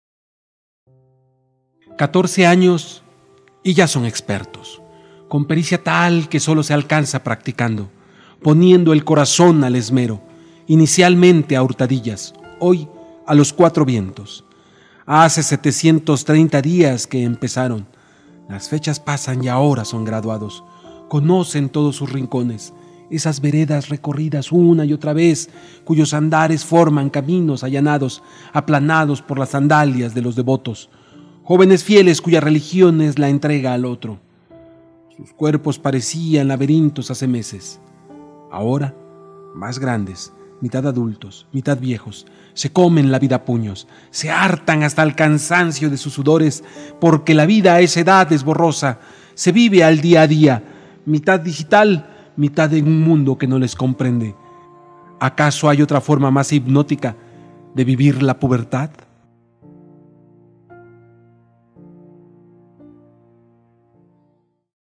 Catorce años. Poema de Abel Pérez Rojas
Catorce años y ya son expertos
con pericia que sólo se alcanza practicando,
poniendo el corazón al esmero,
inicialmente a hurtadillas,
hoy a los cuatro vientos.

Abel Pérez Rojas (n. en Tehuacán, Puebla, México; 6 de enero de 1970) poeta, comunicador, académico y gestor de espacios de educación permanente presenciales y virtuales.